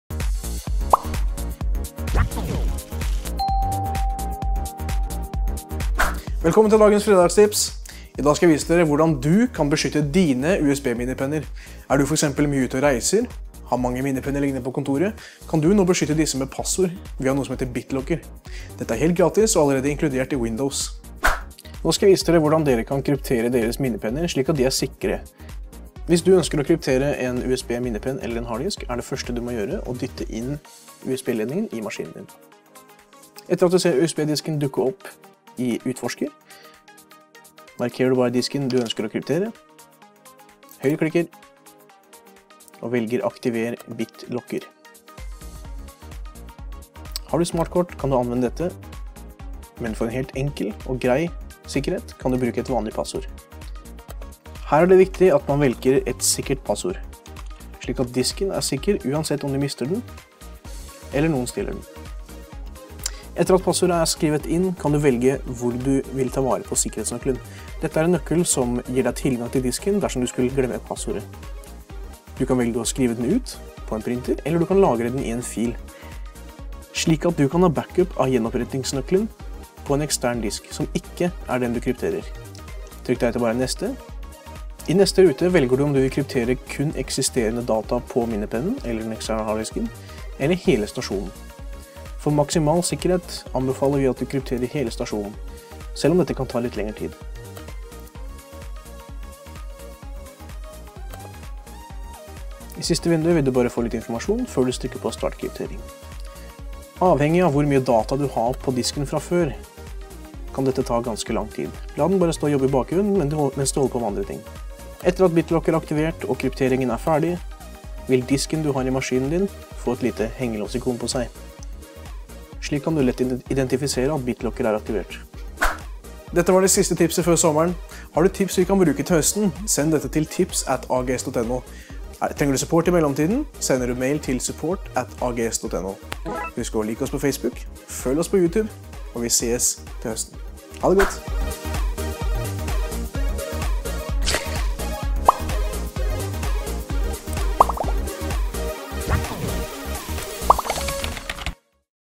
Kryptering av minnepinne eller USB disk i Windows med BitLocker
Nå kan du enkelt sikre og kryptere dine data på USB disk eller minnepenn GRATIS ved å bruke BitLocker funksjonen i Windows. Se hvordan du gjør det i denne videoen.

I dette fredagstipset skal du få se hvordan du kan kryptere en ekstern disk eller en minnepenn. Da kan du være trygg på at informasjonen du har lagret på disse er sikker dersom noen skulle finne på å stjele minnepennen, eller at du kanskje er uheldig og mister den. Ikke minst sikrer du enheten slik at ingen kan lese eller få tak i informasjonen som du har på den. Det beste av alt er at dette er en GRATIS funksjon som heter Bitlocker og som du finner i Windows 8.1. og Windows 10. Det betyr at du kan siker dine eksterne lagringsenheter GRATIS ved å bruke denne funksjonen som du finner innebygget i Windows. Smart ikke sant?

Les hele artikkelen her på vår blogg.